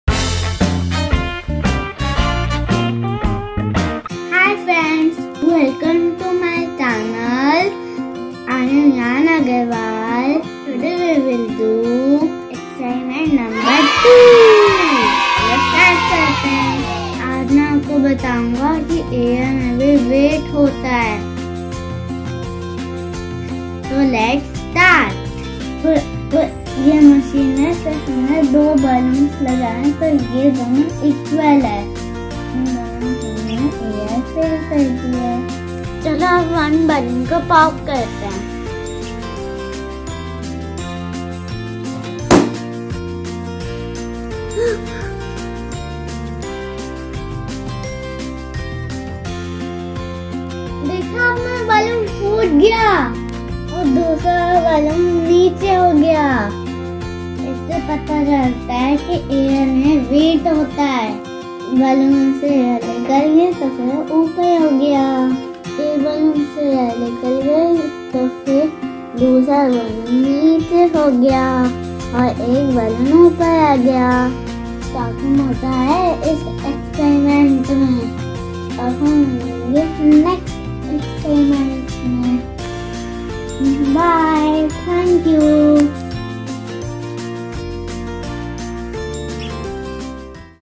Science Experiment# 2 by 4 years old || Air has weight
Science is all about experimenting and learning. So this is the Part # 2 of science experiments done by Viaan. This experiment is to show that Air has weight. For this experiment we have made a weighing machine and have used two air filled balloons.